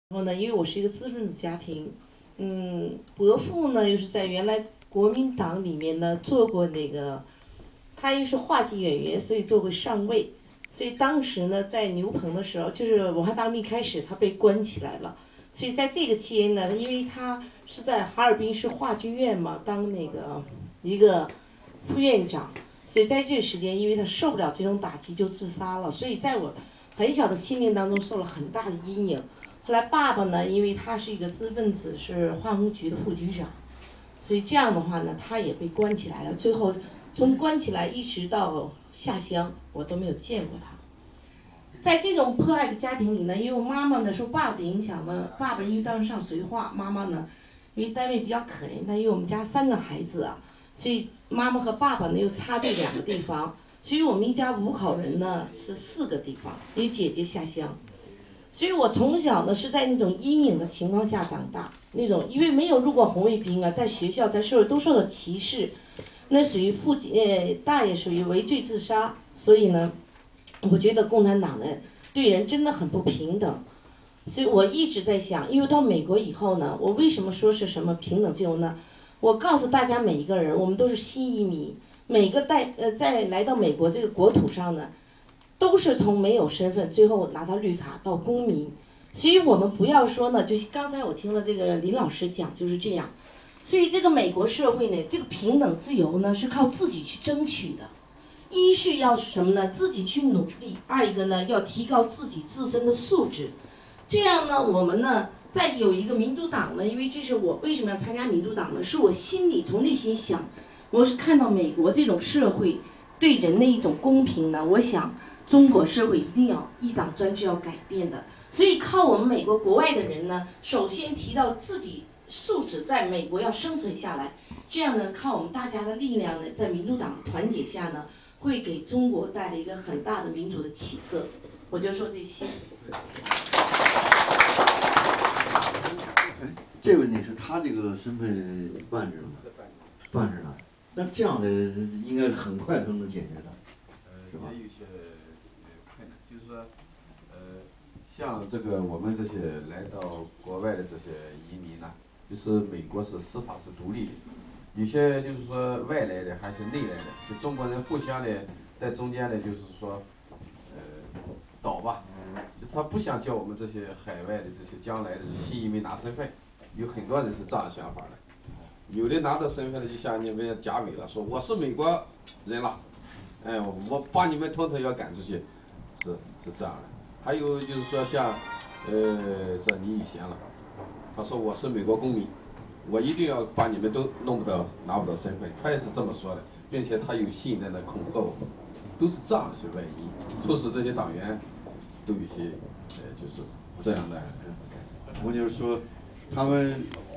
中国民主党 党员言论 china democracy party US headquarter 党员发言
林希翎、任畹町座谈会，党员发言诉说心声，回忆文革中的创伤